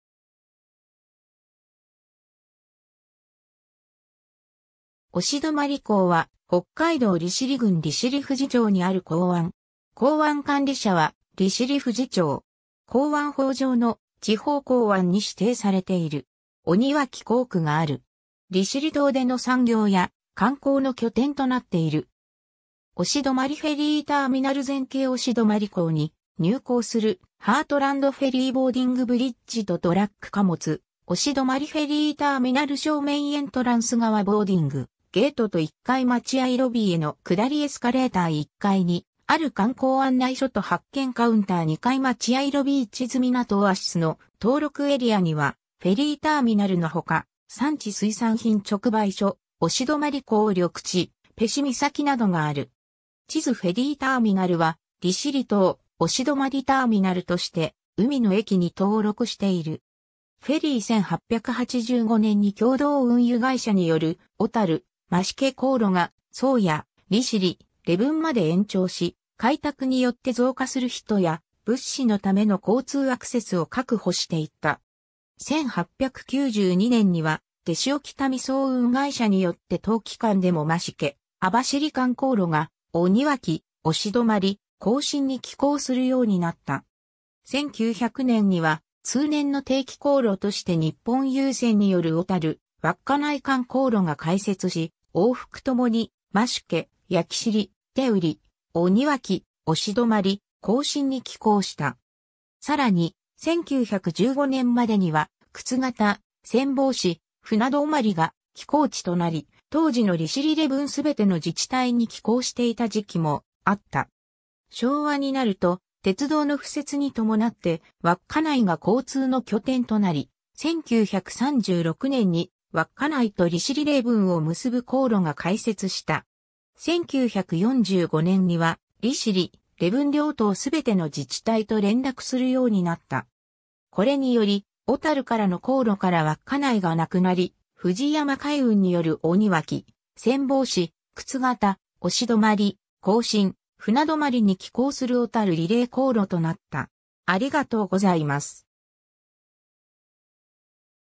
鴛泊港
鴛泊港（おしどまりこう）は、北海道利尻郡利尻富士町にある港湾。
港湾管理者は利尻富士町。
港湾法上の「地方港湾」に指定されている。
鬼脇港区（おにわきこうく）がある。
利尻島での産業や観光の拠点となっている。
 鴛泊フェリーターミナル全景 鴛泊港に入港するハートランドフェリー ボーディングブリッジとトラック貨物 鴛泊フェリーターミナル正面エントランス側 ボーディングゲートと1F待合ロビーへの下りエスカレーター 1Fにある観光案内所と発券カウンター 2F待合ロビー 地図 みなとオアシスの登録エリアにはフェリーターミナルのほか、産地水産品直売所、鴛泊港緑地、ペシ岬などがある。
 地図 フェリーターミナルは「りしりとう・おしどまりターミナル」として「海の駅」に登録している。
 フェリー 1885年（明治18年）に共同運輸会社による小樽—増毛航路が宗谷、利尻、礼文まで延長し、開拓によって増加する人や物資のための交通アクセスを確保していった。
1892年（明治25年）には、天塩北見漕運会社によって冬期間でも増毛—網走間航路が鬼脇、鴛泊、香深に寄港するようになった。
1900年（明治33年）には通年の定期航路として日本郵船による小樽—稚内間航路が開設し、往復ともに増毛、焼尻、天売、鬼脇、鴛泊、香深に寄港した。
さらに、1915年（大正4年）までには沓形、仙法志、船泊が寄港地となり、当時の利尻礼文すべての自治体に寄港していた時期もあった。
昭和になると鉄道の敷設に伴って稚内が交通の拠点となり、1936年（昭和11年）に稚内と利尻礼文を結ぶ航路（稚内利礼航路）が開設した。
1945年（昭和20年）には利尻・礼文両島すべての自治体と連絡するようになった。
これにより小樽からの航路から稚内がなくなり、藤山海運（後に北海商船が継承）による鬼脇、仙法志、沓形、鴛泊、香深、船泊に寄港する小樽利礼航路となった。